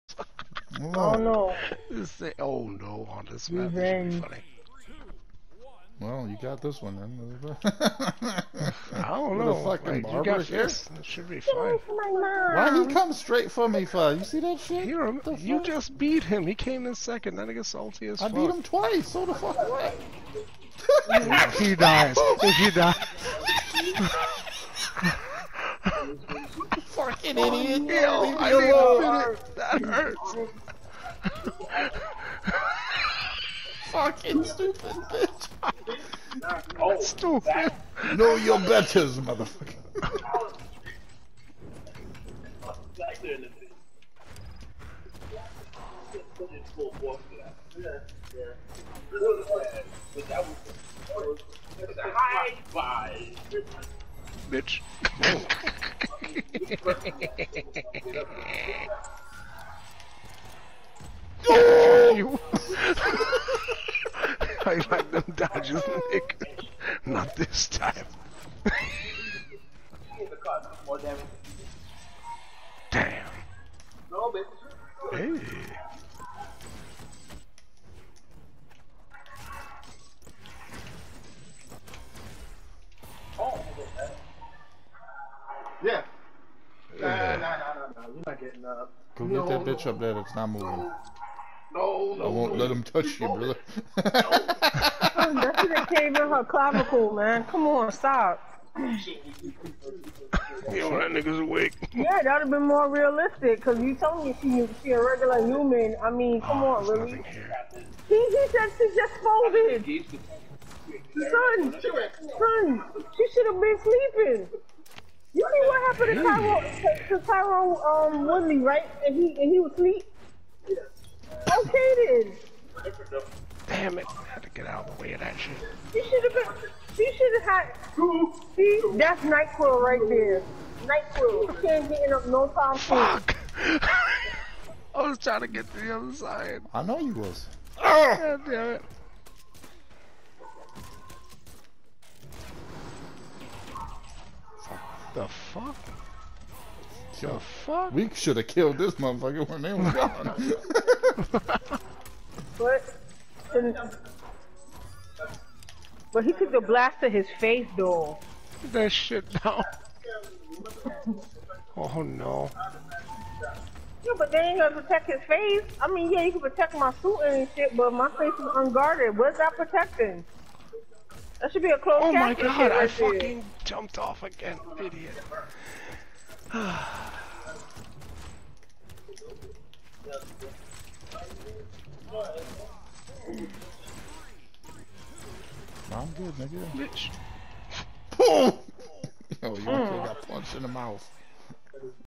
Stop Chasing Me
The Vraxx was in 4-5 games in a row with us, and got beat up every time.